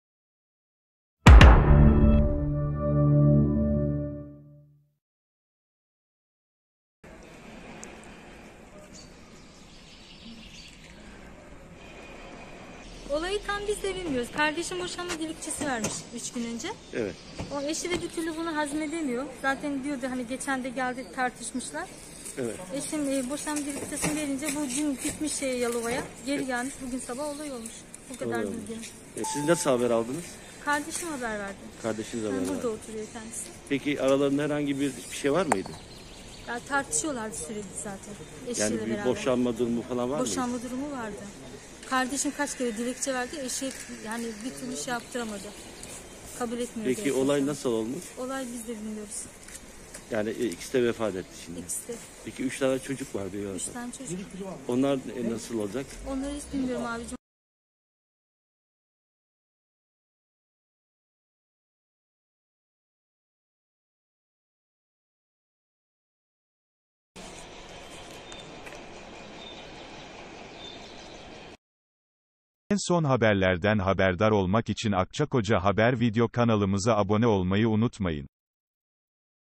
Düzce nin Akçakoca ilçesinde Cinnet getiren şahıs önce karısını, ardından kendini öldürdü
Düzce’nin Akçakoca ilçesinde sabah saatlerinde eşi tarafından çocuklarının gözü önünde öldürülen kadının ablası, “Sürekli tartışıyorlardı. Kardeşim kaç kere boşanma dilekçe verdi” dedi.

Düzce’nin Akçakoca ilçesi Osmaniye Mahallesi Çuhallı Çarşısı'nda sabah saatlerinde 49 yaşındaki Metin Aygün eşi Mediha Aygün ile tartıştı.

Tartışmanın büyümesi sonucunda Metin Aygün çocuklarının gözü önünde bıçak ile önce karısını öldürdü sonda da kendi canına kıydı.

Cenazeler ise otopsileri yapılmak üzere Akçakoca Devlet Hastanesi morguna kaldırılırken yakınlarının bekleyişi sürüyor.

Öldürülen kadının ablası Sema Yıldız olayı kardeşinden duyduğunu belirterek, “Sürekli tartışıyorlardı.

Kardeşim kaç kere boşanmak için dilekçe verdi. Eşi bir türlü kabul etmedi.

Olayı tam olarak bizde bilmiyoruz. Kardeşim 3 gün önce boşanma dilekçesi vermişti. Eşi bunu bir türlü hazmedemiyor.

Geçen geldiğinde tartışmışlar. Dün Yalova’ya gitmiş. Bu sabah gelmiş. Bu olay olmuş” şeklinde konuştu.

Olayla ilgili olarak soruşturma devam ediyor.